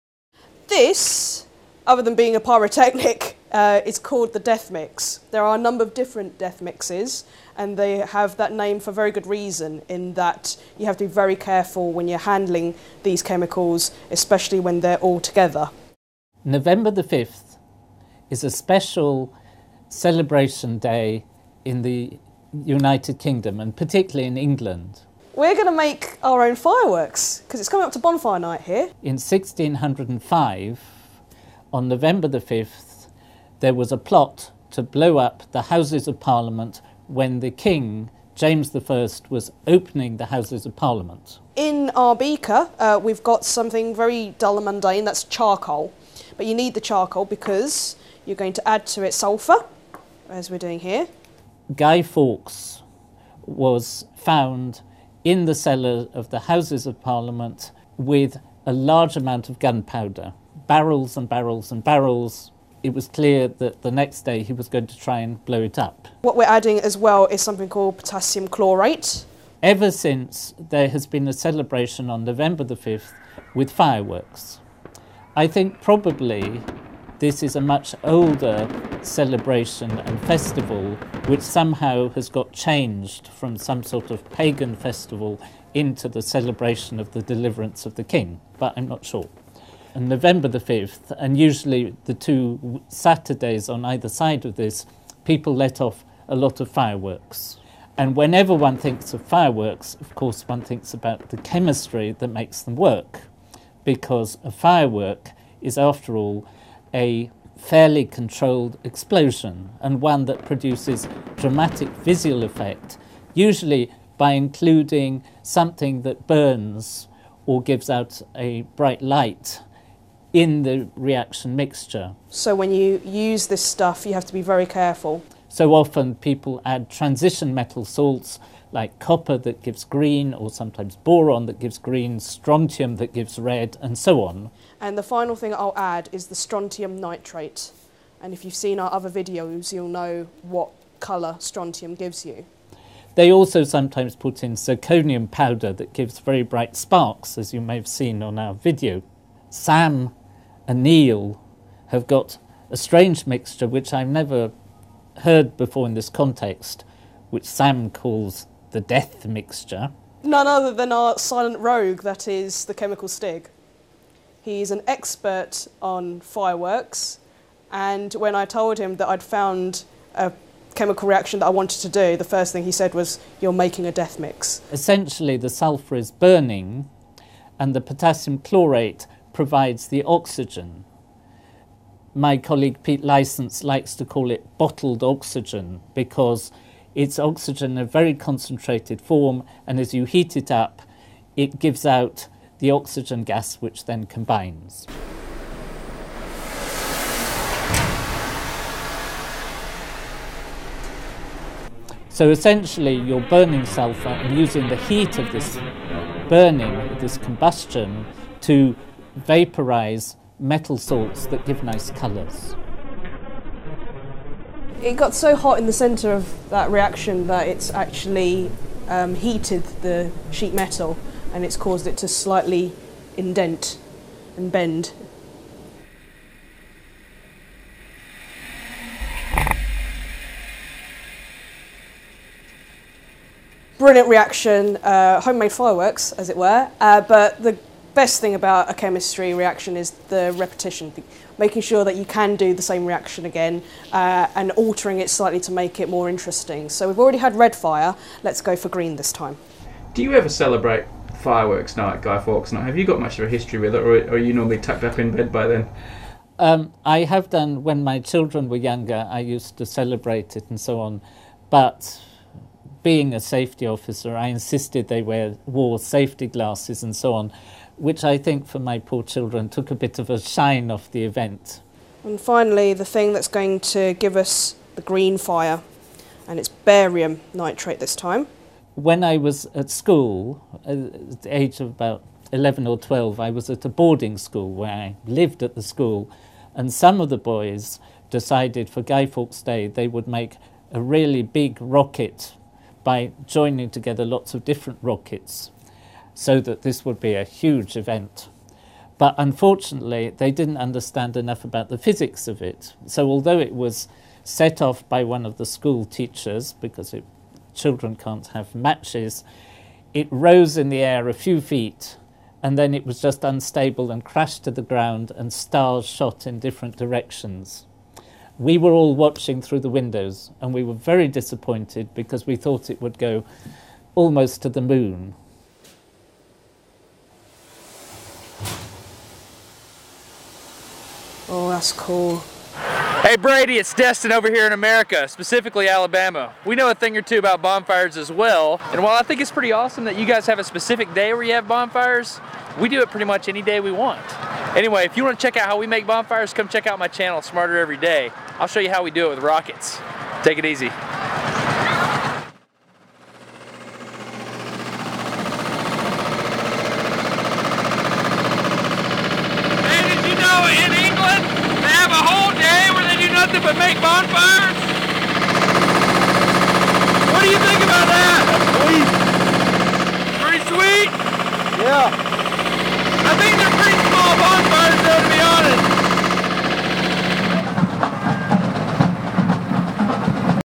Death Mix and Guy Fawkes Night - Periodic Table of Videos
Guy Fawkes Night in the UK is a time for bonfires and fireworks. Here's some "death mix" to shed light on things.

- it's worth a look.